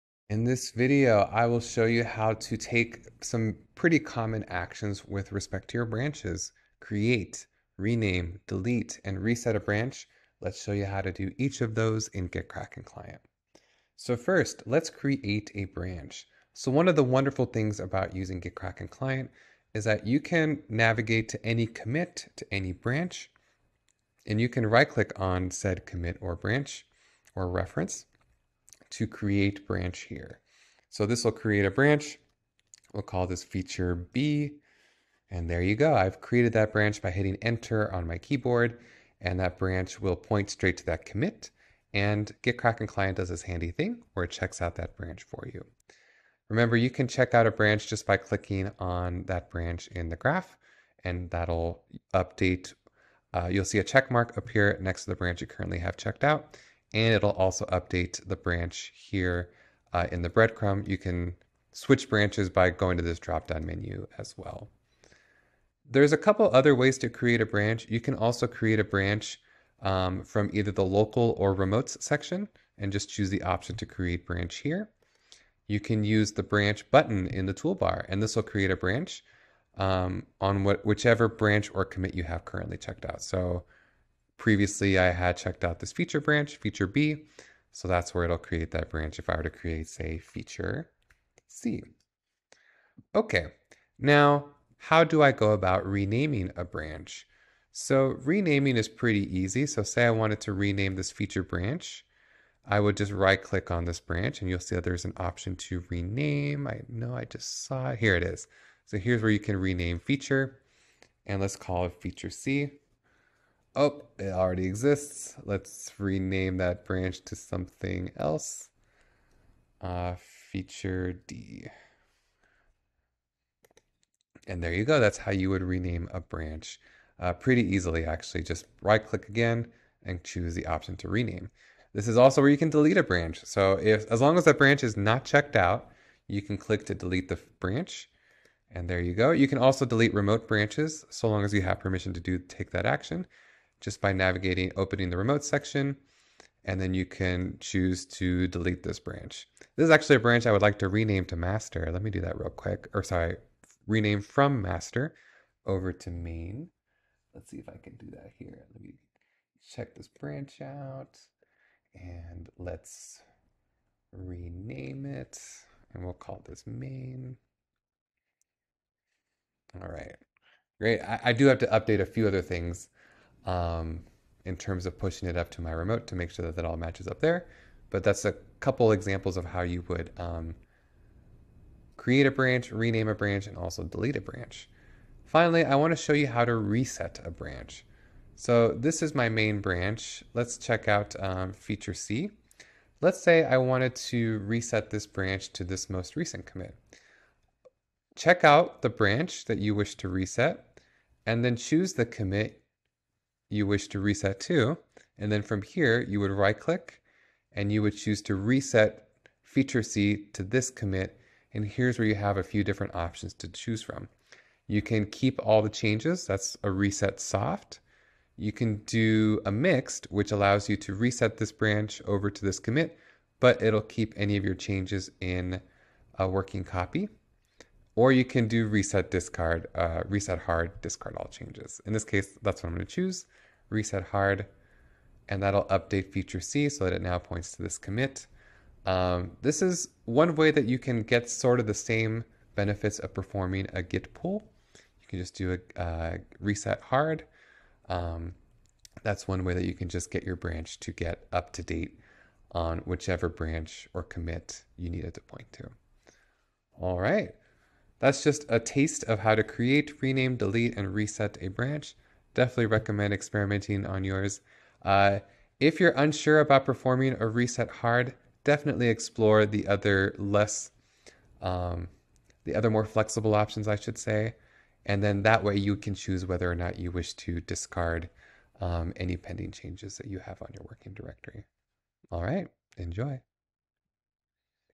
GitKraken Desktop 101 Ep 25: Manage Branches | Rename Without Regret
Branch management made simple

In this GitKraken Desktop tutorial, you'll learn how to create, rename, delete, and reset branches without touching the command line. Use the visual commit graph to manage your workflow faster and more confidently.

What you'll learn:
• How to create a new branch
• How to rename or delete a branch
• How to reset a branch to a specific commit